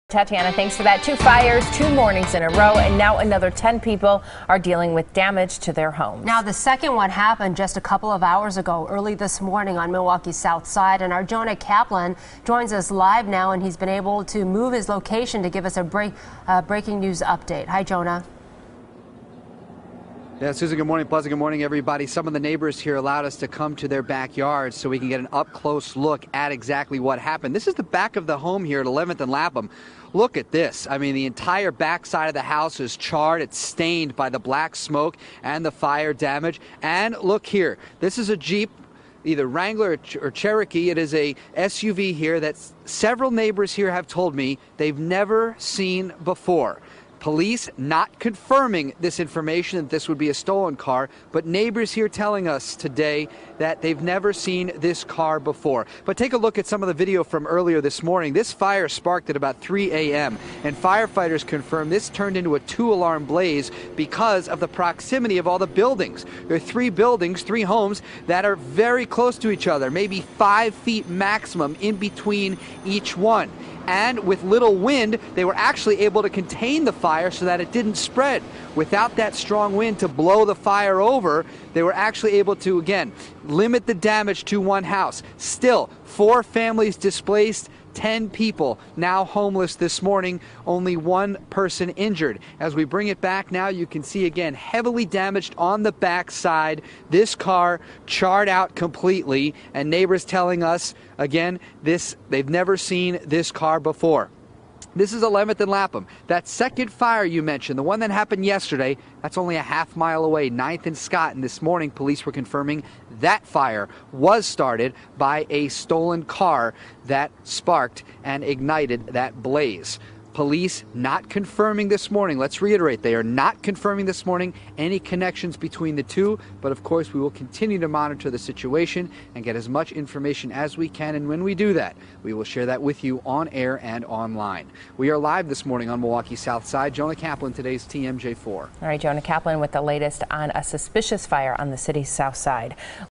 Police investigate 'suspicious fire' on Milwaukee's south side
MILWAUKEE -- Milwaukee police are investigating a "suspicious fire" that broke out around 3 a.m. Thursday near 11th Street and Lapham Boulevard on the city's south side.